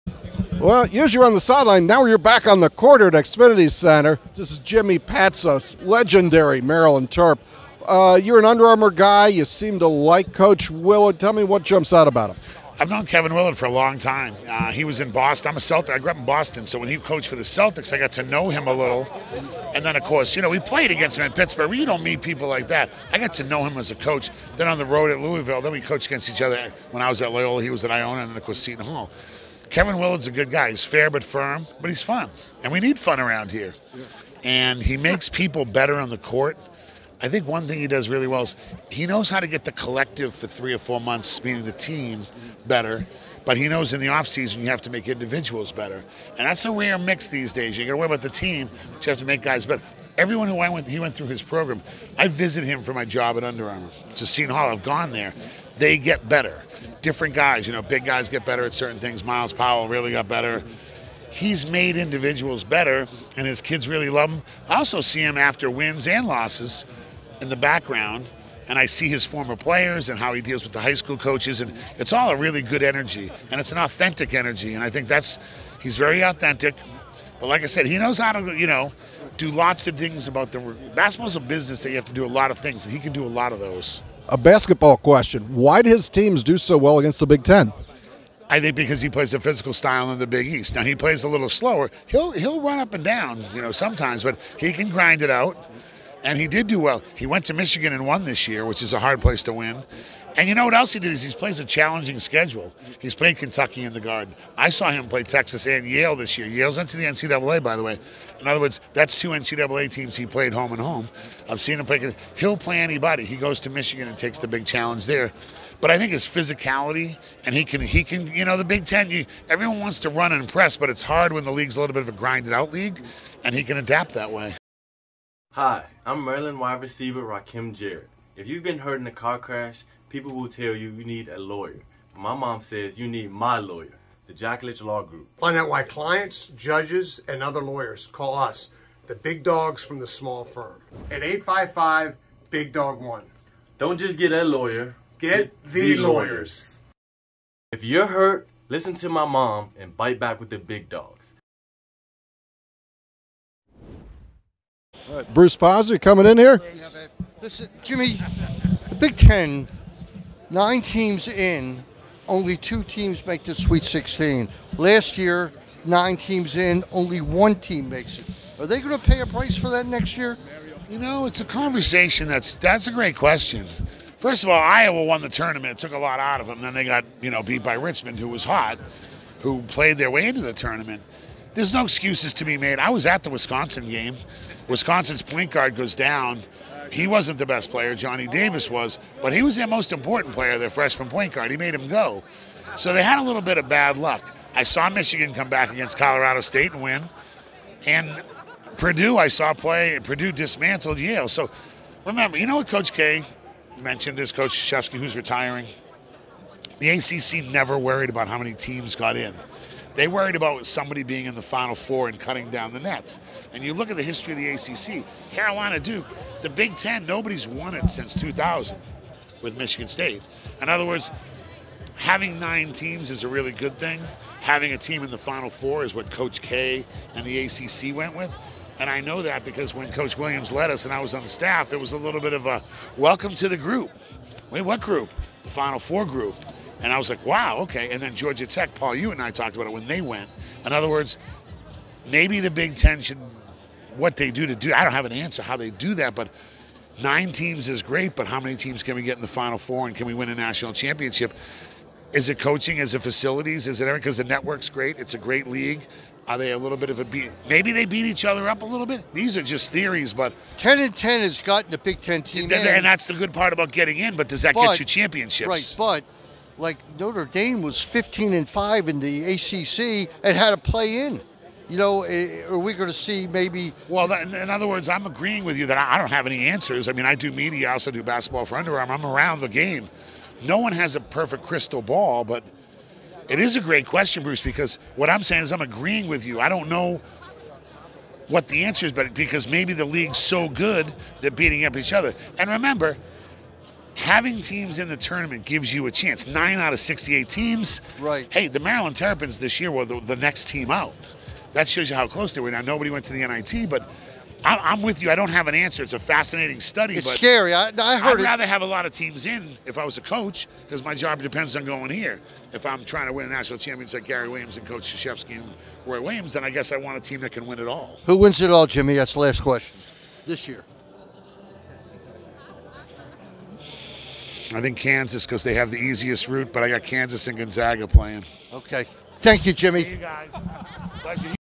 Terptalk w Jimmy Patsos after Kevin Willard presser
Kevin Willard is the new Maryland head basketball coach.  Jimmy Patsos chats with Wayne Viener and Bruce Posner about the choice.